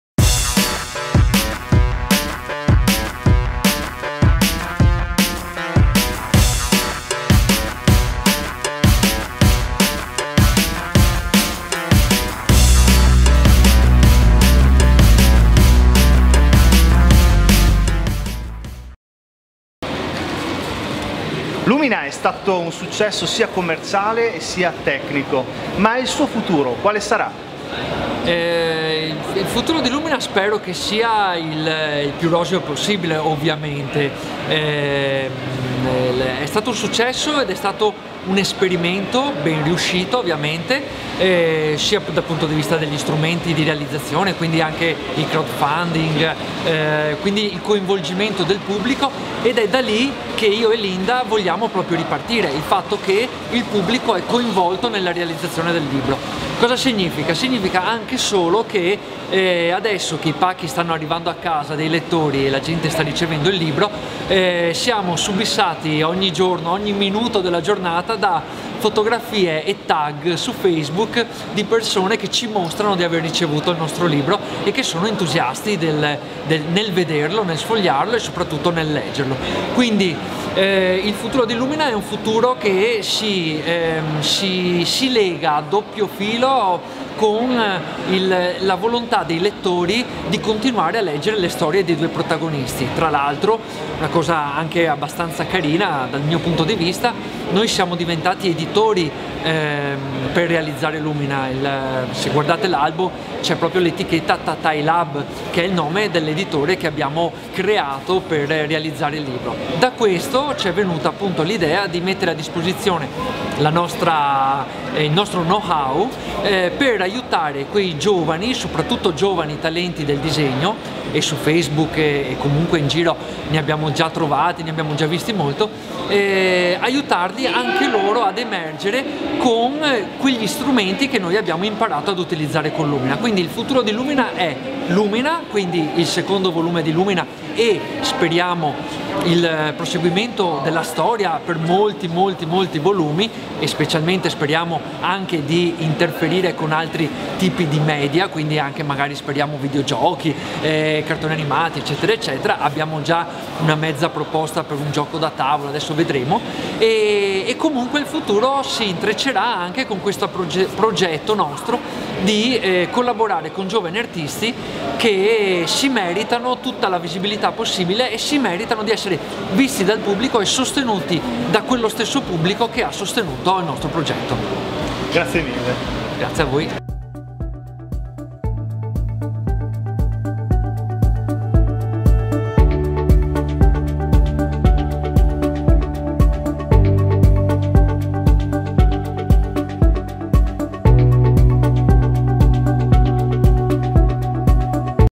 Intervista A Emanuele Tenderini
Quale sarà il futuro prossimo del progetto Lumina? Scopriamolo assieme a Emanuele Tenderini, autore del fumetto assieme a Linda Cavallini.